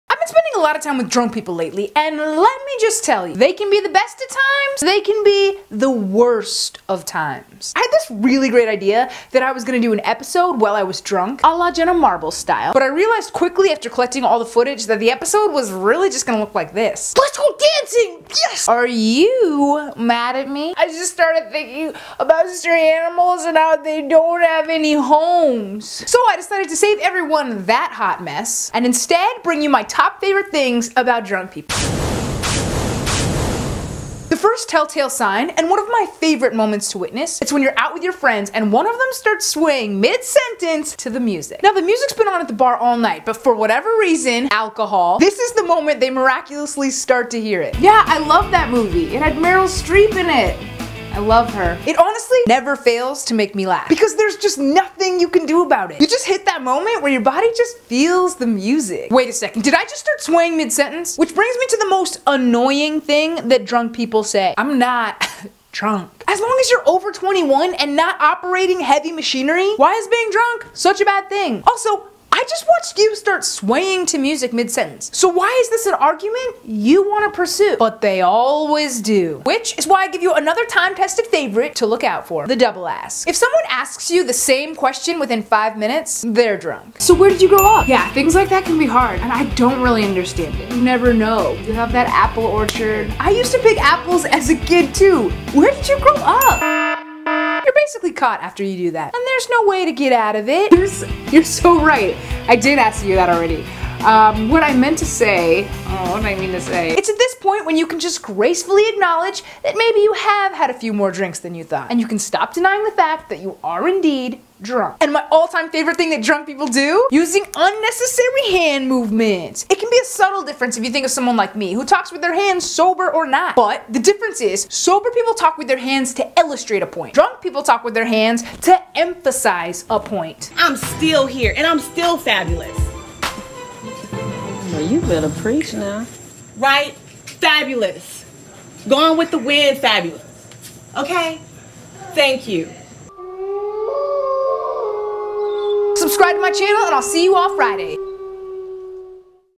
S#!T Drunk People Say!
S#!T drunk people say and do. I make fun of drunk people and all the crazy things they do and say. I also teach you how to tell if some one is drunk. Their are always tell tale signs and you should probably know them so you dont get tricked into thinking someone is sober. Cause then you wont make fun of them. Making fun of your friends when they are drunk and bringing up stupid things they did the last time they were drunk is a staple to best friend ship!  ...or im told...